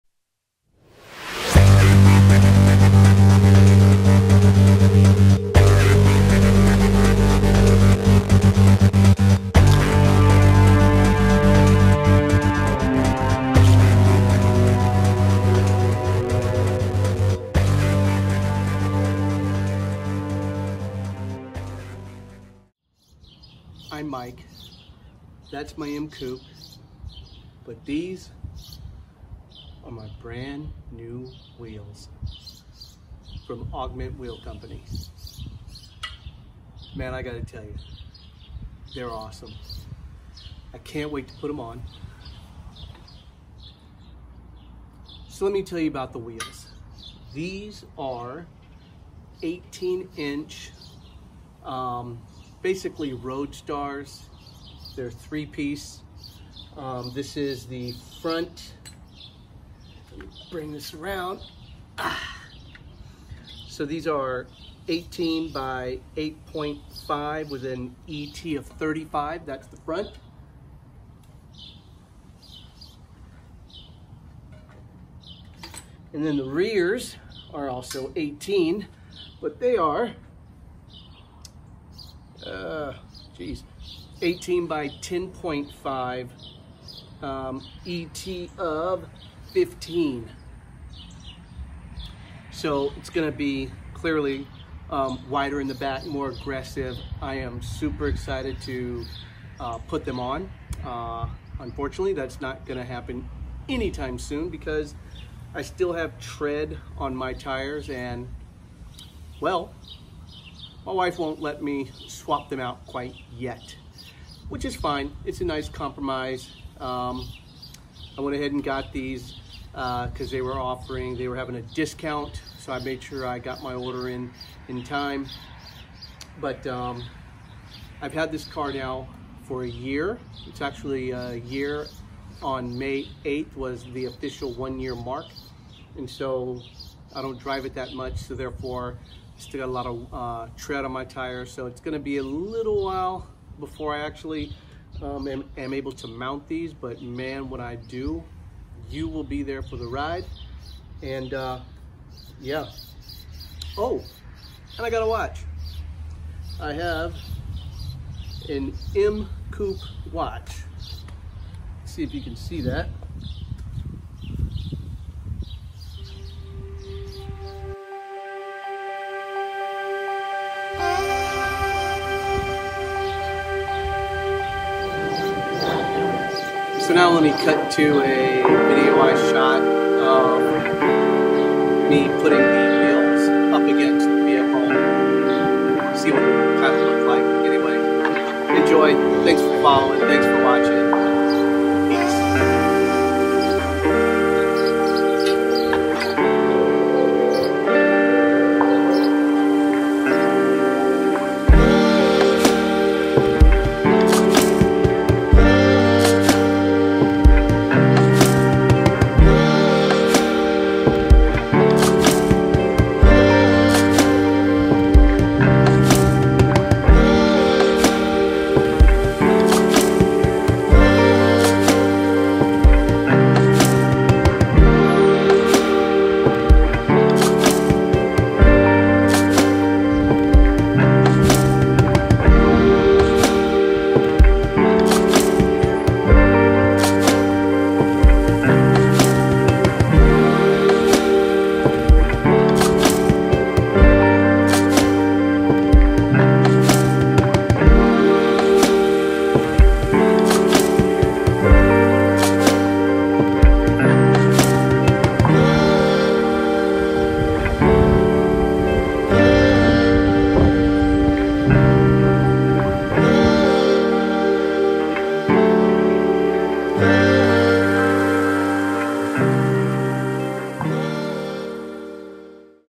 NEW WHEELS for Mike's ///M Coupe!
I recently got my new 18" 3-piece roadstar style wheels from Augment Wheel company out of Canada and I love them and can't wait to put them on